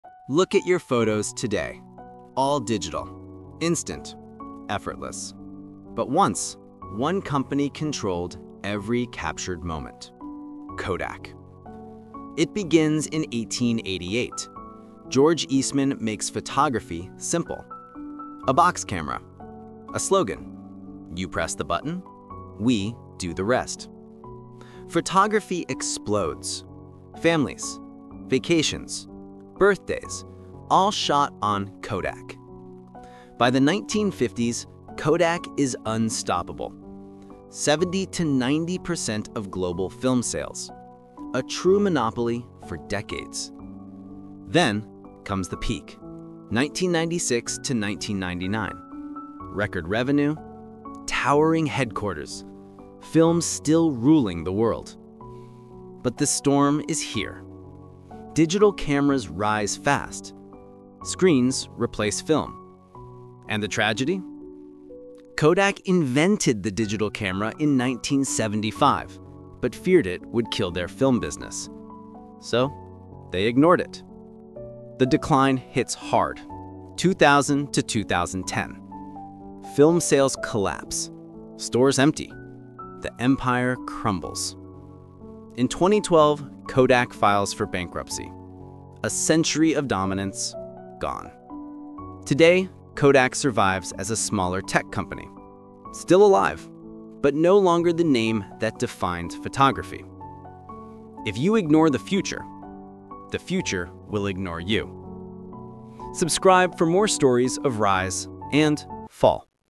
Kodak: The Rise and Sudden Fall
Kodak once controlled 90% of the film market… but they ignored the digital revolution they invented. This is the shocking 20-second story of how a giant collapsed. Perfect for tech, business, and startup audiences.